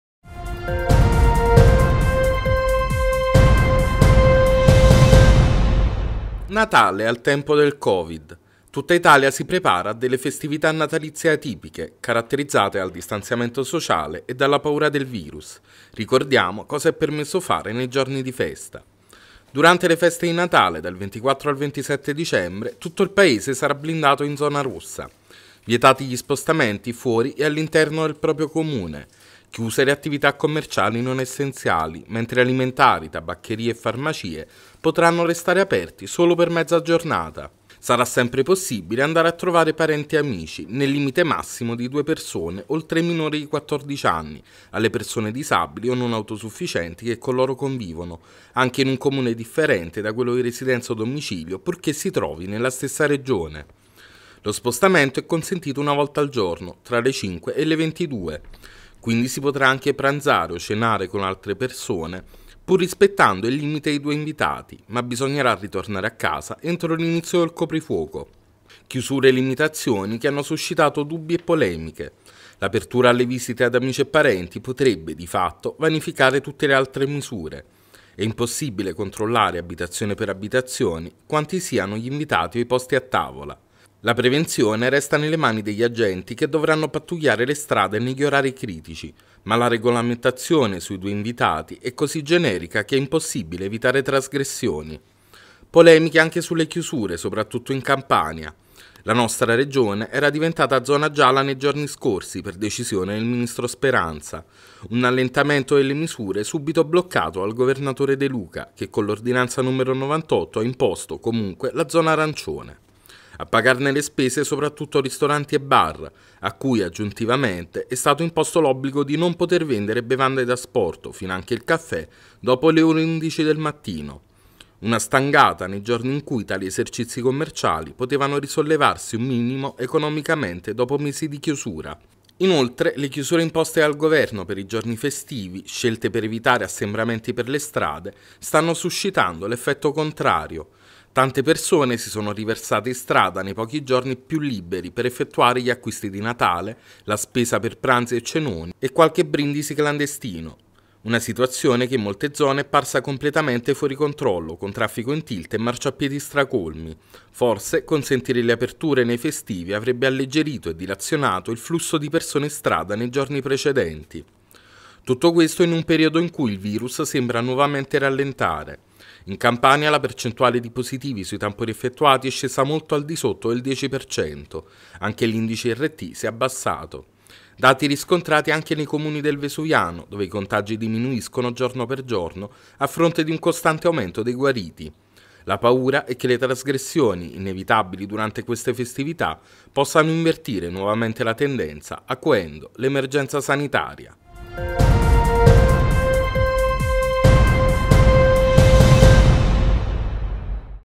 Natale al tempo del Covid: restrizioni, chiusure e tante polemiche - VIDEO
Natale al tempo del Covid. Tutta Italia si prepara a delle festività natalizie atipiche, caratterizzate dal distanziamento sociale e dalla paura del virus. Ricordiamo cosa è permesso fare nei giorni di festa.
Durante le feste di Natale, dal 24 al 27 dicembre, tutto il paese sarà blindato in zona rossa. Vietati gli spostamenti fuori e all’interno del proprio Comune. Chiuse le attività commerciali non essenziali, mentre alimentari, tabaccherie e farmacie potranno restare aperti solo per mezza giornata. Sarà sempre possibile andare a trovare parenti e amici, nel limite massimo di due persone oltre ai minori di 14 anni, alle persone disabili o non autosufficienti che con loro convivono, anche in un comune differente da quello di residenza o domicilio, purché si trovi nella stessa Regione. Lo spostamento è consentito una volta al giorno, tra le 5 e le 22. Quindi si potrà anche pranzare o cenare con altre persone, pur rispettando il limite di due invitati, ma bisognerà ritornare a casa entro l’inizio del coprifuoco.
Chiusure e limitazioni che hanno suscitato dubbi e polemiche. L’apertura alle visite ad amici e parenti potrebbe, di fatto, vanificare tutte le altre misure. E’ impossibile controllare, abitazione per abitazione, quanti siano gli invitati o i posti a tavola. La prevenzione resta nelle mani degli agenti che dovranno pattugliare le strade negli orari critici, ma la regolamentazione sui due invitati è così generica che è impossibile evitare trasgressioni.
Polemiche anche sulle chiusure, soprattutto in Campania. La nostra regione era diventata zona gialla nei giorni scorsi, per decisione del ministro Speranza. Un allentamento delle misure subito bloccato dal Governatore De Luca, che con l’ordinanza n. 98 ha imposto, comunque, la zona arancione. A pagarne le spese soprattutto ristoranti e bar, a cui, aggiuntivamente, è stato imposto l’obbligo di non poter vendere bevande d’asporto, finanche il caffè, dopo le ore 11 del mattino. Una stangata nei giorni in cui tali esercizi commerciali potevano risollevarsi un minimo economicamente dopo mesi di chiusura.
Inoltre, le chiusure imposte dal Governo per i giorni festivi, scelte per evitare assembramenti per le strade, stanno suscitando l’effetto contrario. Tante persone si sono riversate in strada nei pochi giorni più “liberi” per effettuare gli acquisti di Natale, la spesa per pranzi e cenoni e qualche brindisi clandestino. Una situazione che in molte zone è parsa completamente fuori controllo, con traffico in tilt e marciapiedi stracolmi. Forse, consentire le aperture nei festivi avrebbe alleggerito e dilazionato il flusso di persone in strada nei giorni precedenti.
Tutto questo in un periodo in cui il virus sembra, nuovamente, rallentare. In Campania la percentuale di positivi sui tamponi effettuati è scesa molto al di sotto del 10%, anche l’indice RT si è abbassato. Dati riscontrati anche nei Comuni del Vesuviano, dove i contagi diminuiscono giorno per giorno a fronte di un costante aumento dei guariti. La paura è che le trasgressioni, inevitabili durante queste festività, possano invertire nuovamente la tendenza, acuendo l’emergenza sanitaria.